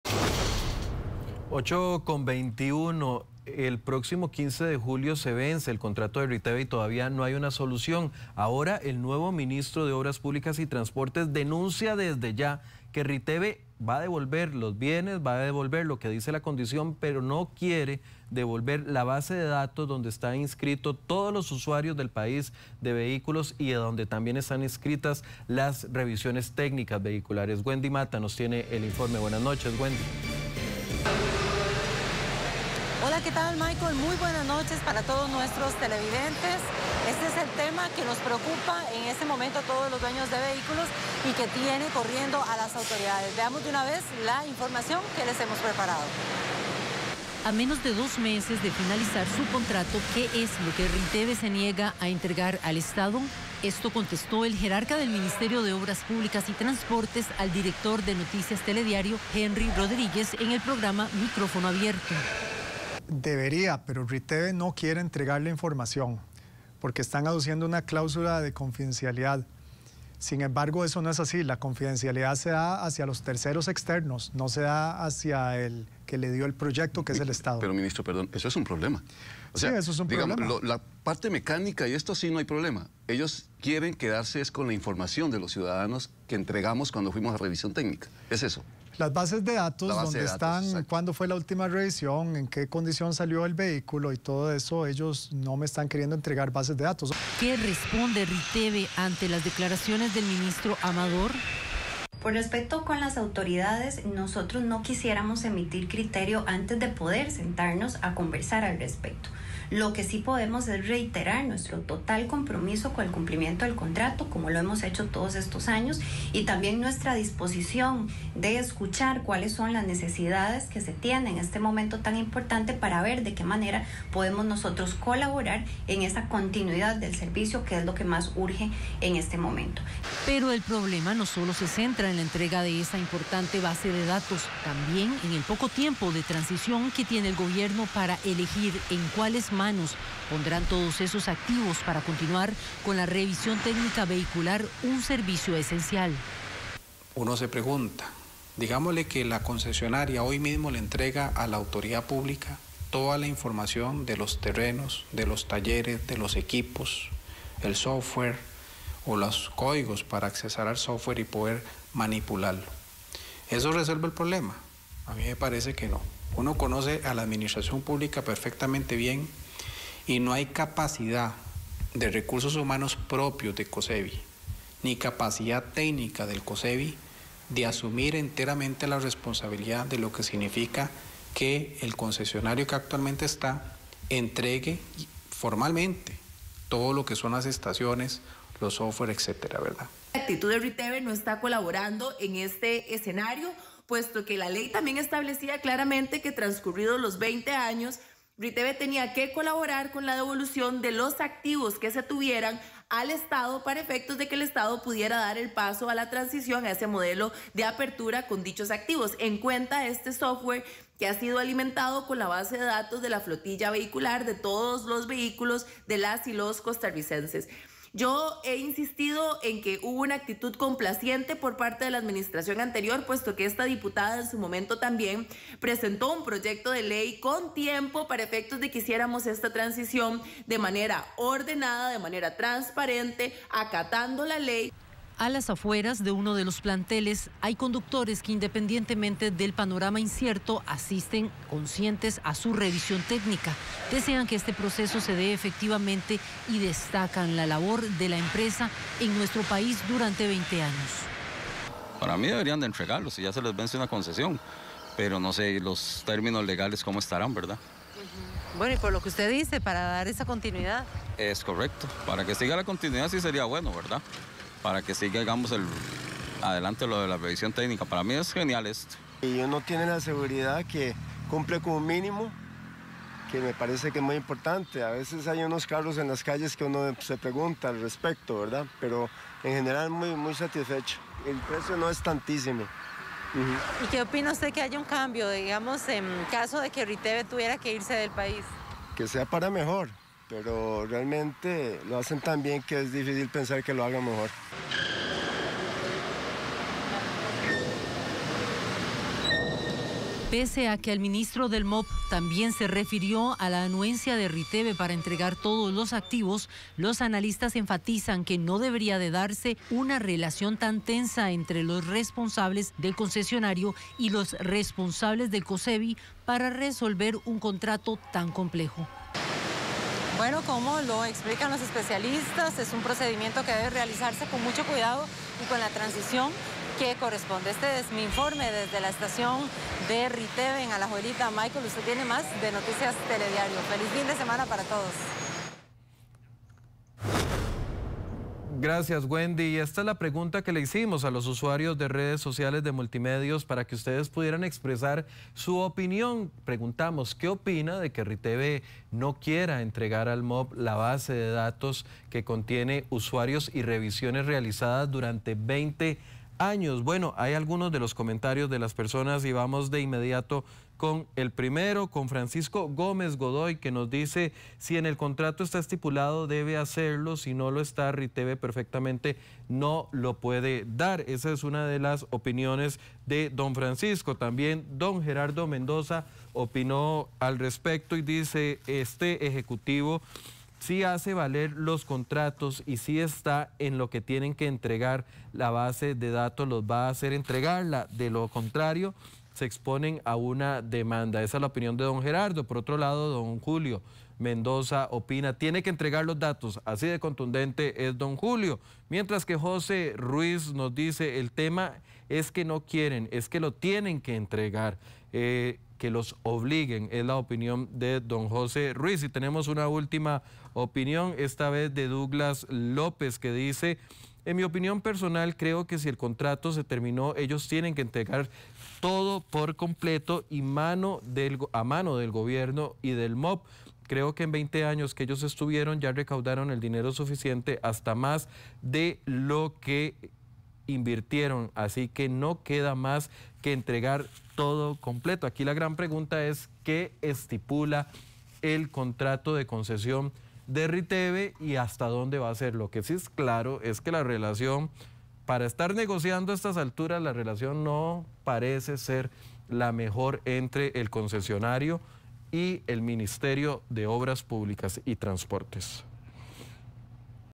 Riteve se niega a entregar base de datos al MOPT
Ministro del MOPT dice que empresa se resiste a dar base de datos